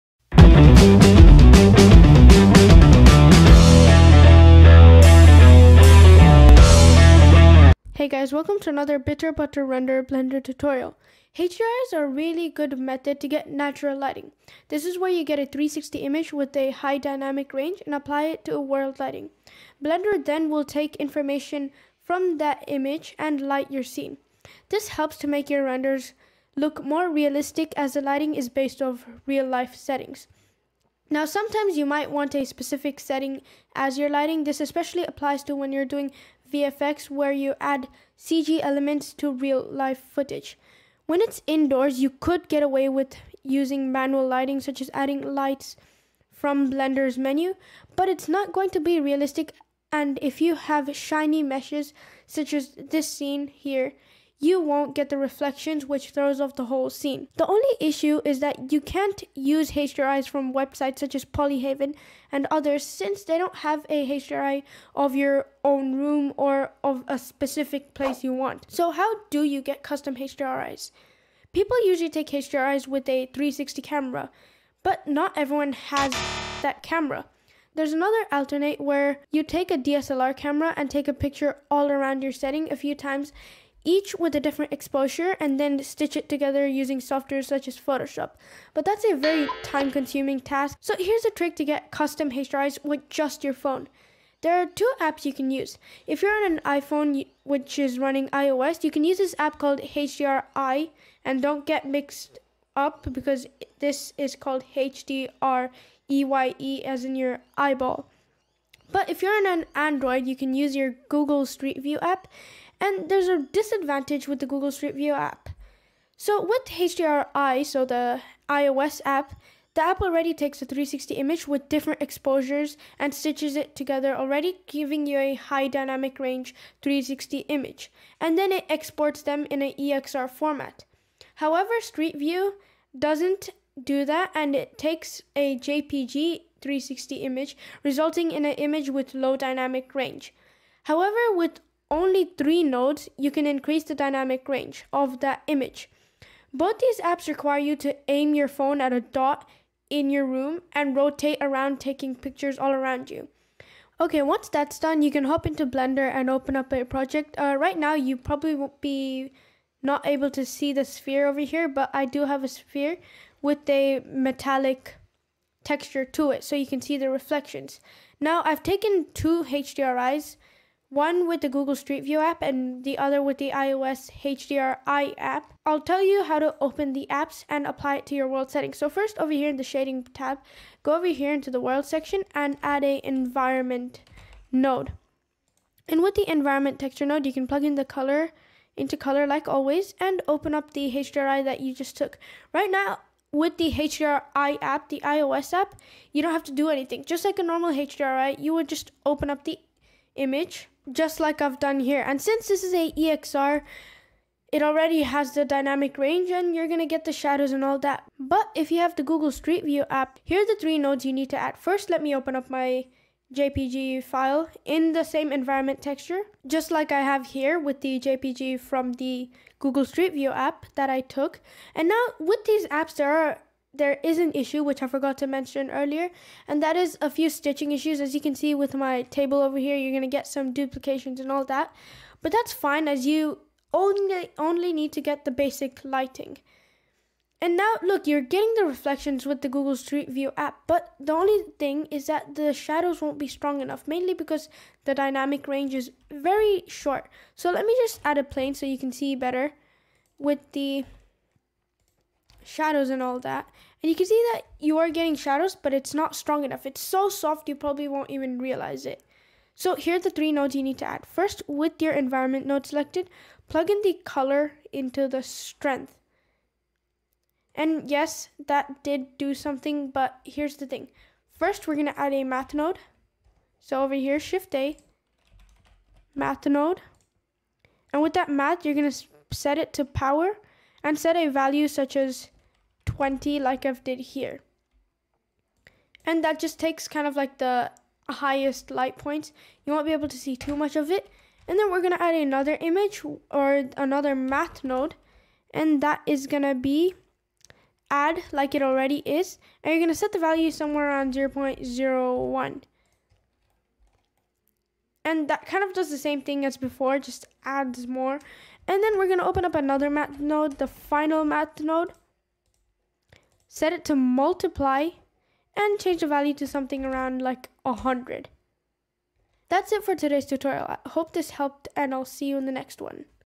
You can now make CUSTOM HDRI's with JUST your phone!
You can now make custom hdri's with just your phone! This is what you will learn in this tutorial.

APPS:
               IOS - Street view: